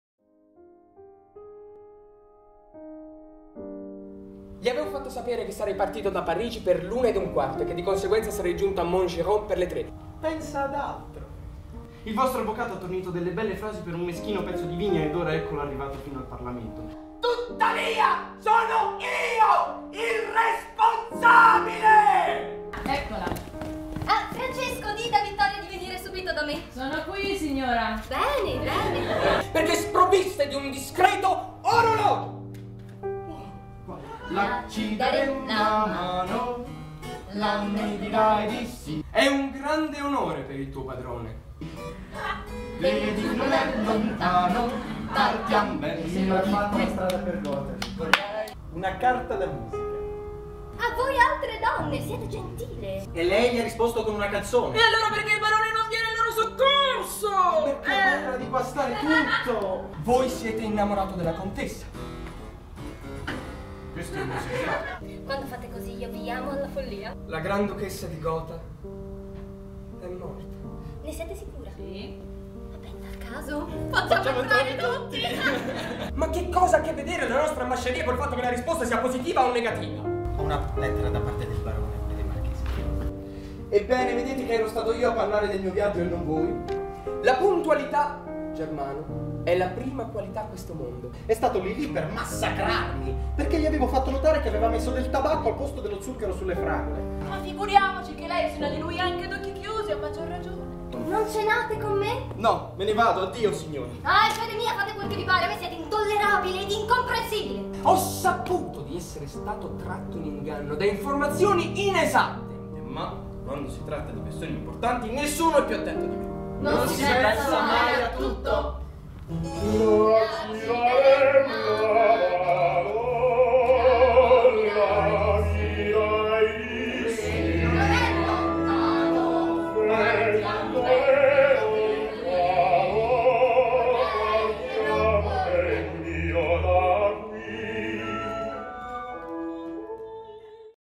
Trailer di "Non si pensa mai a tutto" Maggio 2014
SAGGIO ALLIEVI EUTHECA III ANNO | RECITAZIONE

Regia Federica TATULLI

COLL' AMORE NON SI SCHERZA e ALTRE COMMEDIE
di Alfred DE MUSSET

Dal 15 al 18 Maggio 2014
Ore 17.00 - Ore 21.00